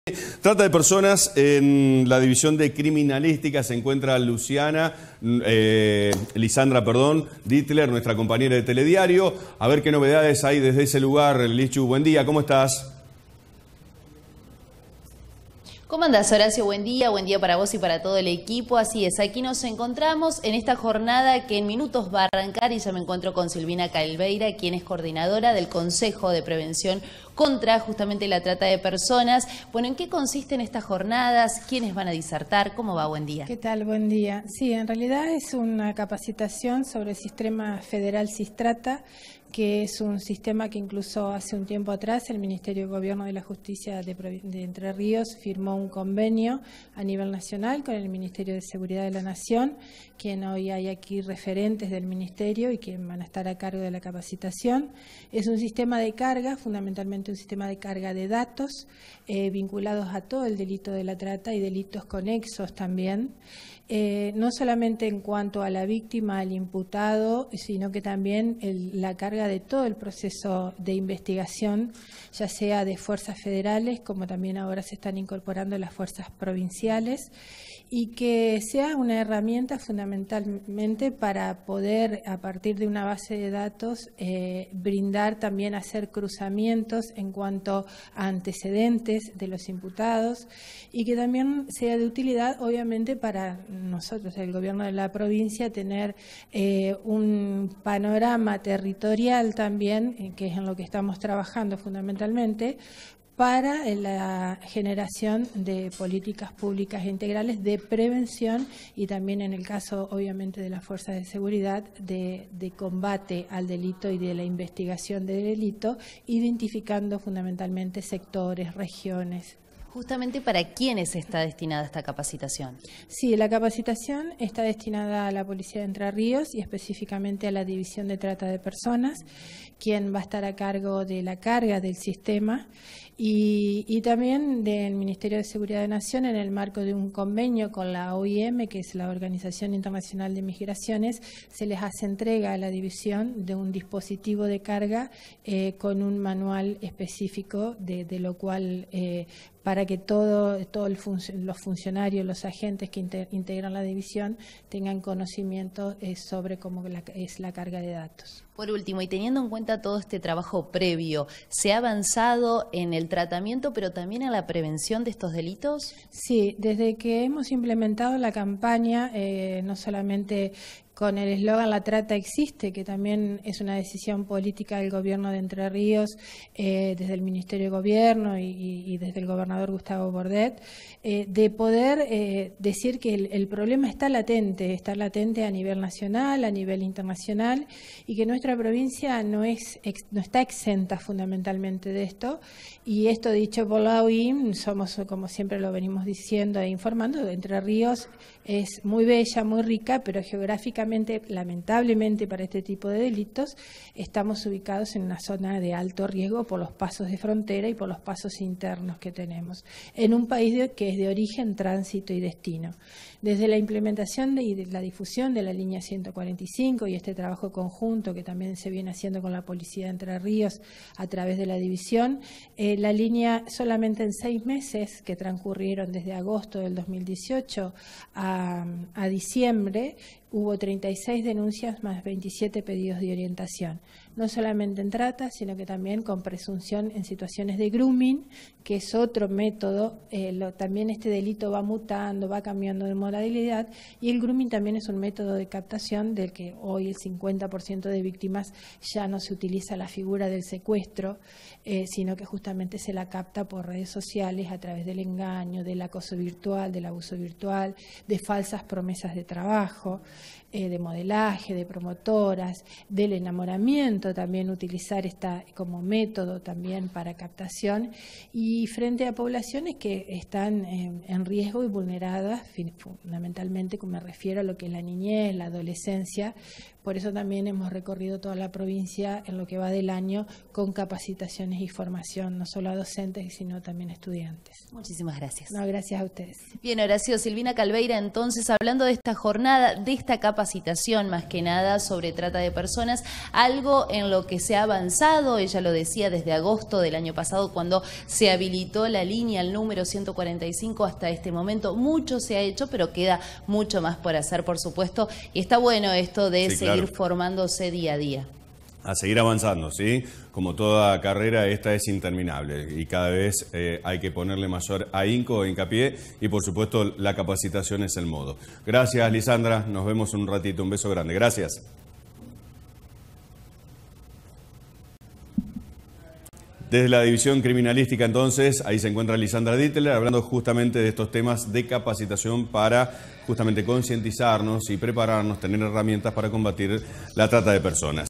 Jornadas sobre trata
Se realizó una jornada sobre la problemática de trata de personas en la división criminalística de la policía de Entre Ríos, Lisandra Ditler nos brindó información desde el lugar.